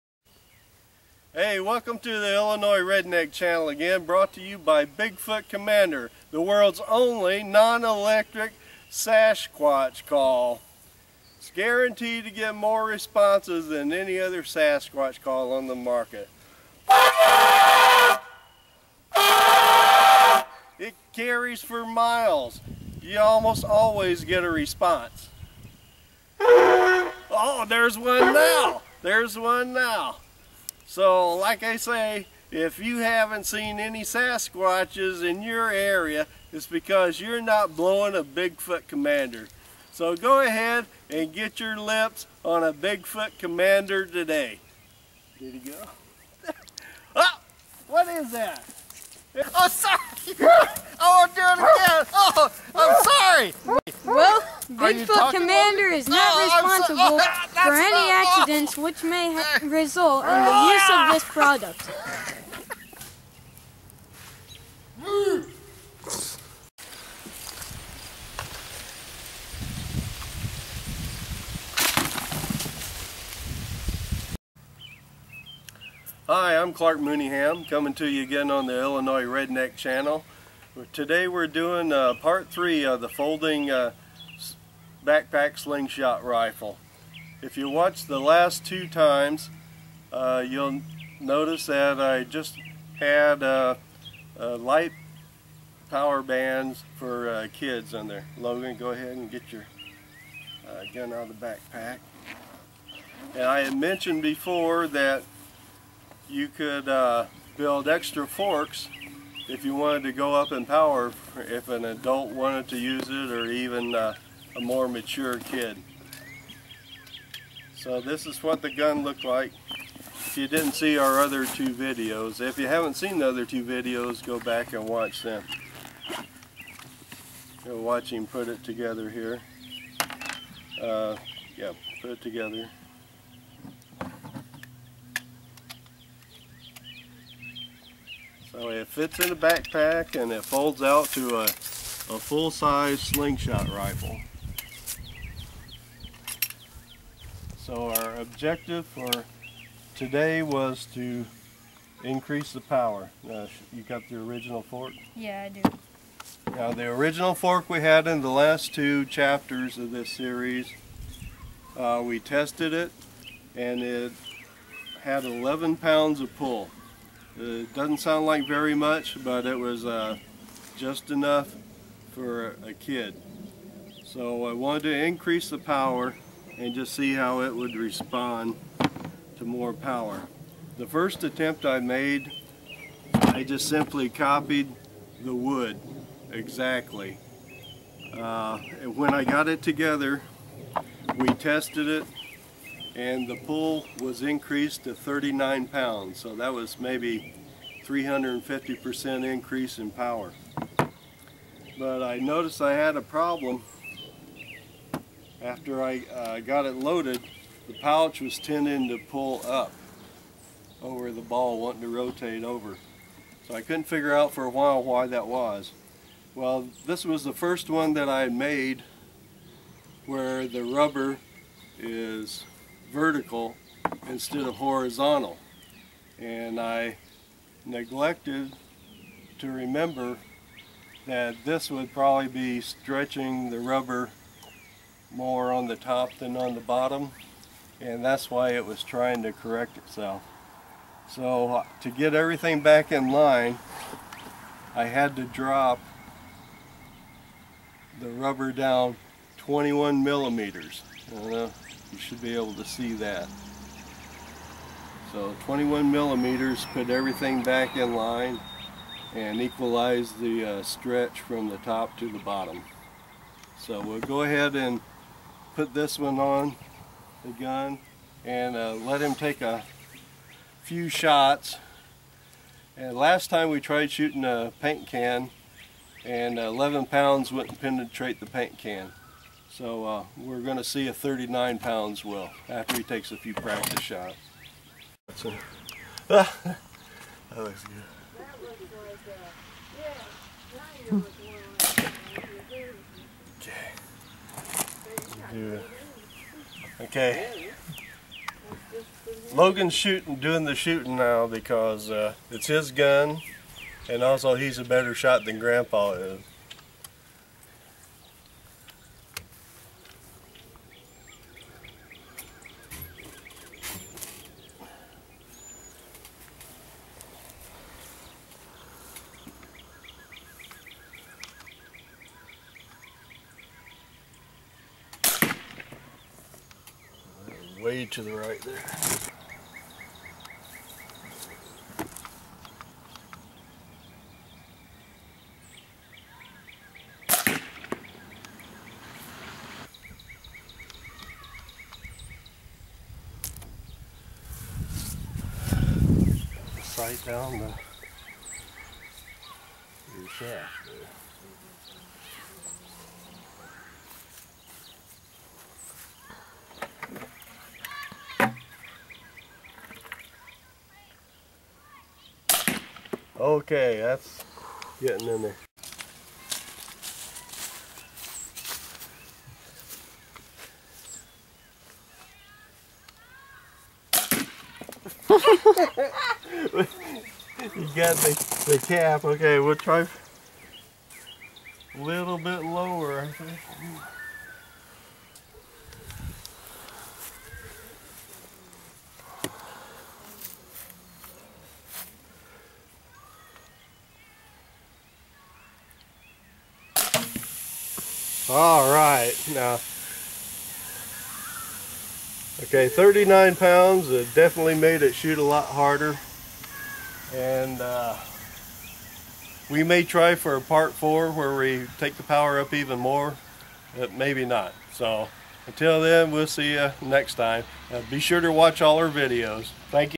Folding Backpack Slingshot Rifle pt. 3
A simple change of fork on the end of the folding backpack slingshot increases the power by 350%; and now we're ready to go sasquatch hunting.  Shot on location at the Bundy Ranch.